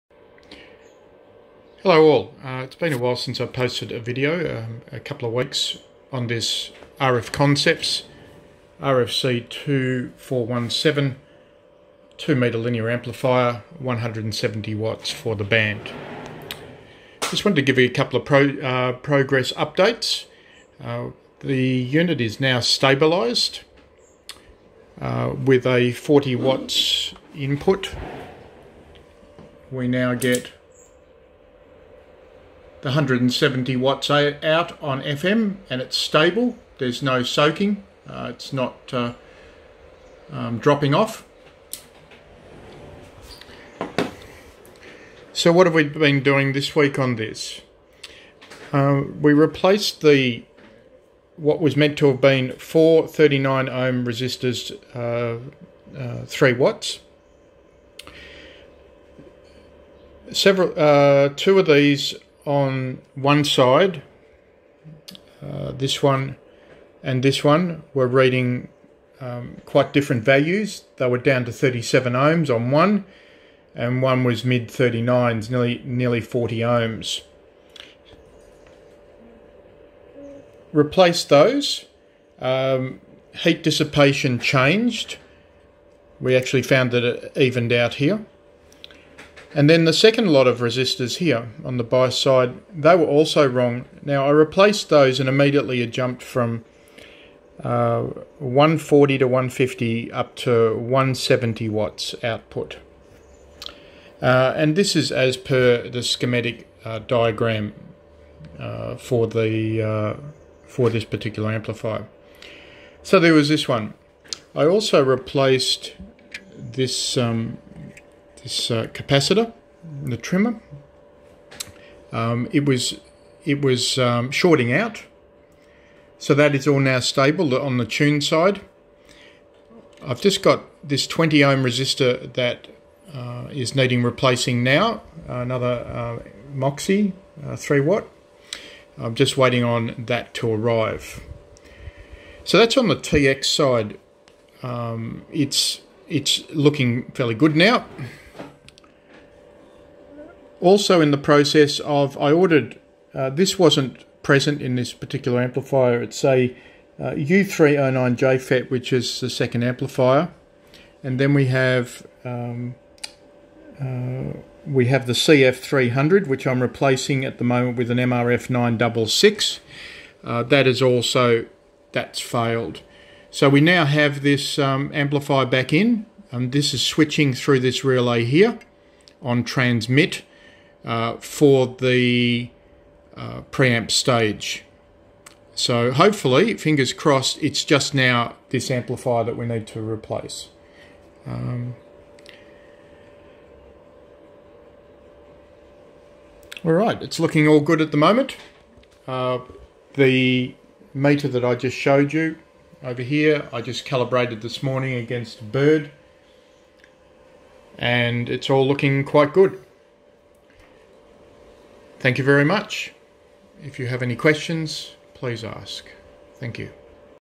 RF Concepts 2-417 2 Meter Amp Restoration Part 4
replaced 4 x resistors
replaced trimmer in tune circuit
added U309 which was not present in the pre amp stage

20ohm resistor to be replaced
mrf966 to replace cf300 transistor in pre amp stage

170w in FM now reliable